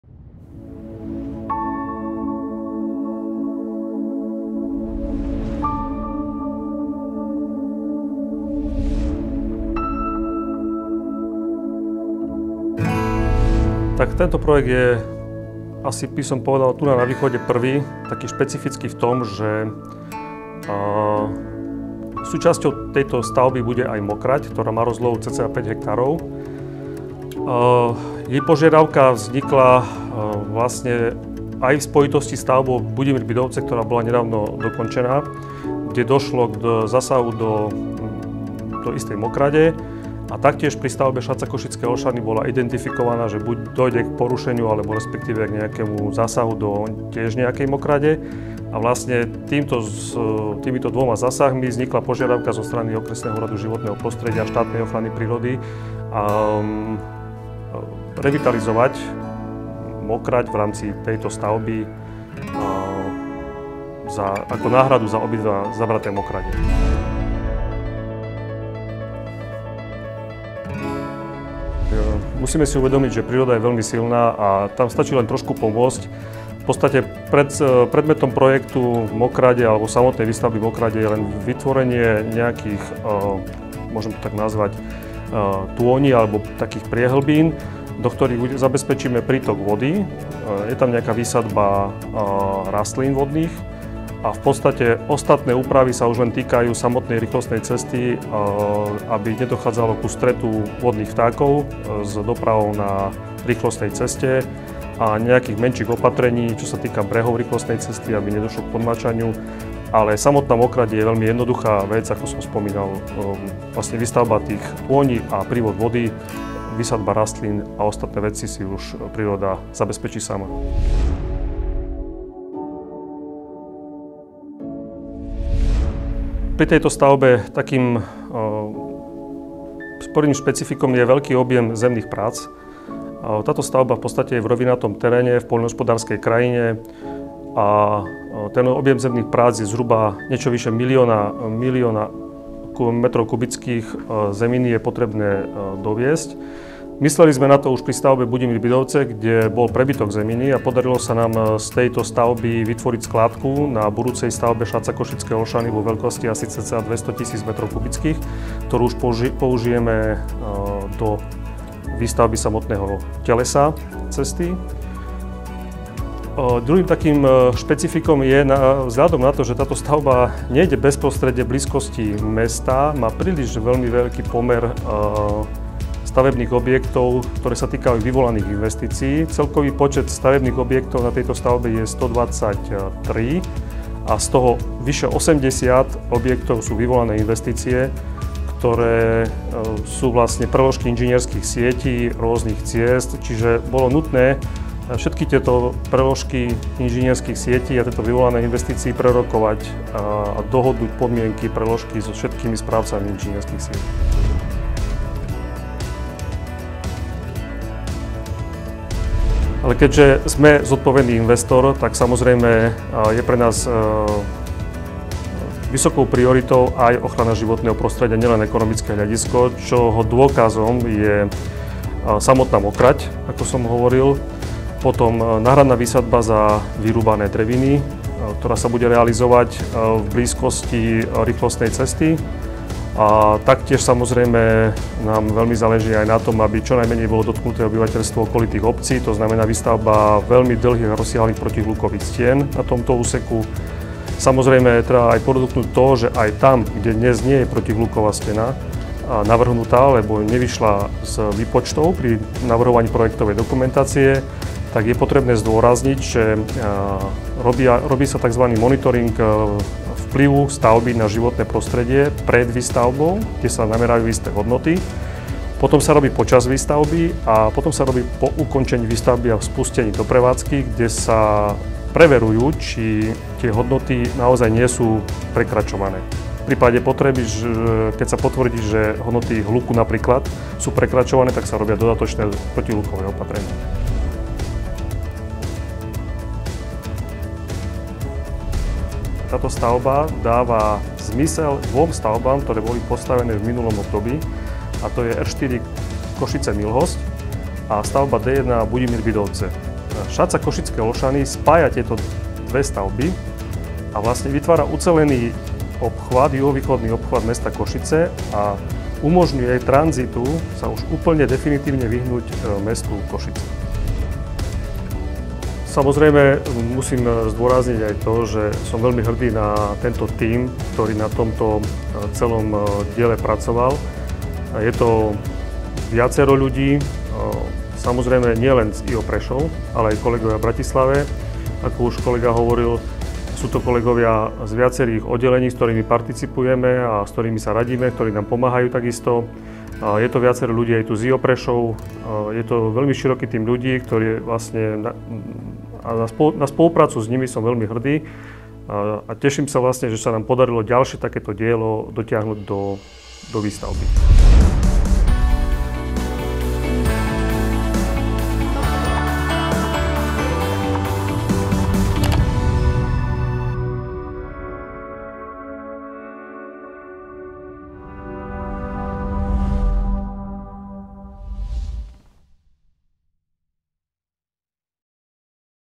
NDS | Povolenia | Košice Naokolo
Dokumentácia pre územné rozhodnutie sme mali hotovú v 11/2013, územné rozhodnutie nadobudlo platnosť v roku 2015. Dokumentáciu pre stavebné povolenie sme prevzali v lete 2018 a stavebné povolenie sme získali v 10/2020. V rámci prípravy sme zrealizovali aj projekt na rekultiváciu mokradí.